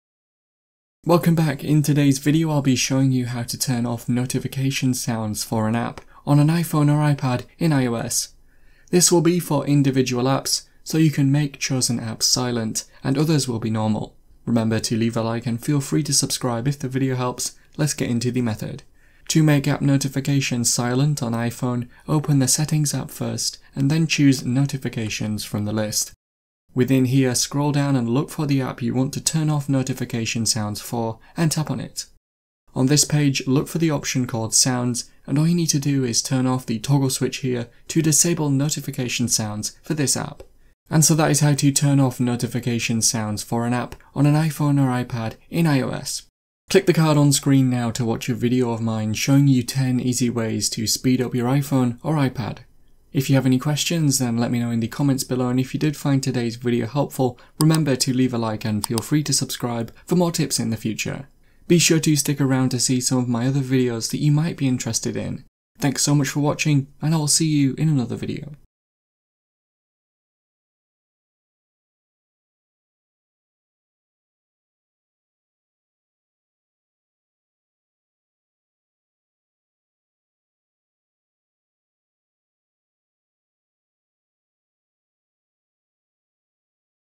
How to Turn Off App Notification Sounds on iPhone or iPad (iOS)
Want to know how to turn off notification sounds for apps on iPhone or iPad in iOS? This video will show you how to stop app notification sounds on iPhone. You might want to know how to make app notifications silent on iPhone to limit distractions. Hopefully today’s video helps show you how to disable app notification sounds on iPhone!

Video Chapters:
00:00 Intro
00:19 Turn Off App Notification Sounds on iPhone
00:41 Outro